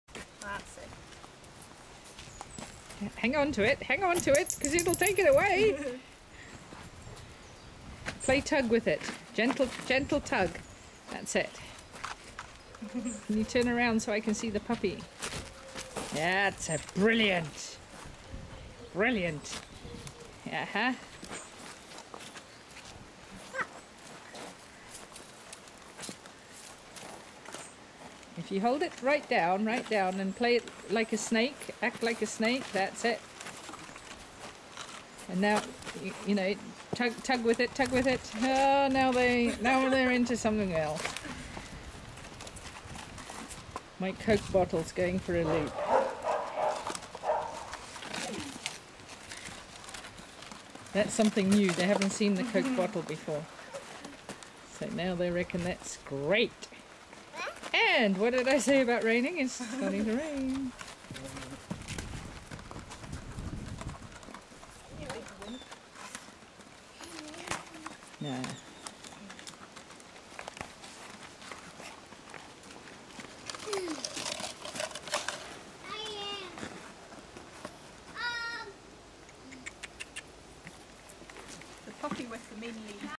Children and pup learning to play tug 20130918
This boy is looking for the right home, 10 weeks old, Male, split face border collie puppy, great with children, loves people, just started his training with treats, looking for a fantastic family environment where he can do agility or herding or just be an active companion.